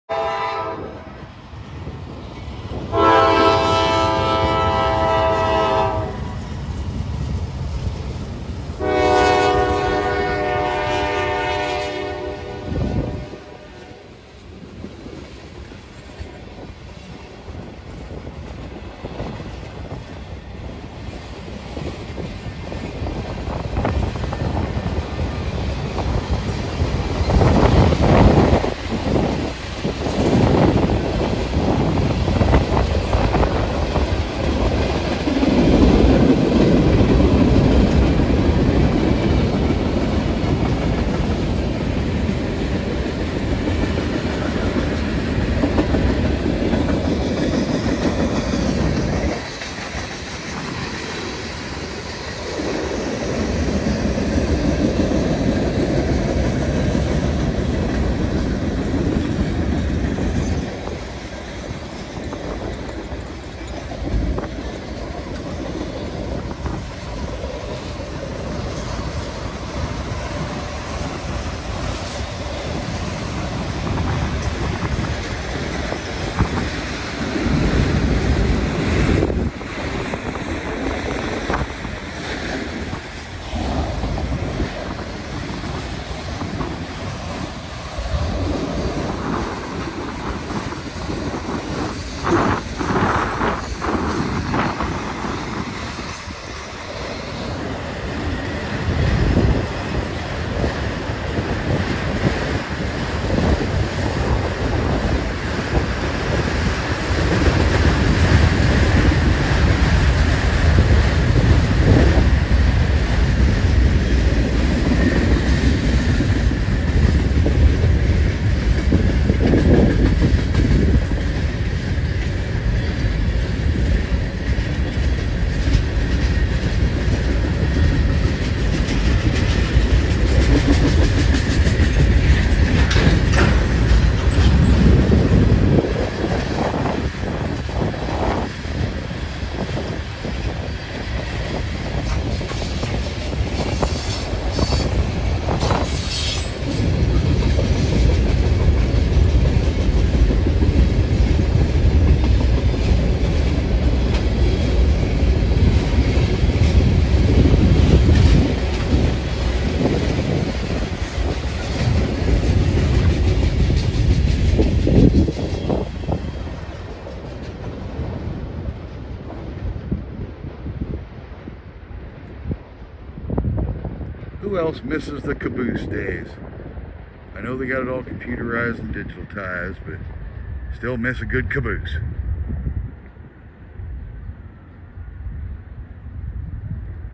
Norfolk & Southern Passing through 2023
As I was checking out the New Transportation Hub Construction a good ole Norfolk & Southern coupled to a Union pacific came rolling through on the last set of old tracks left in place..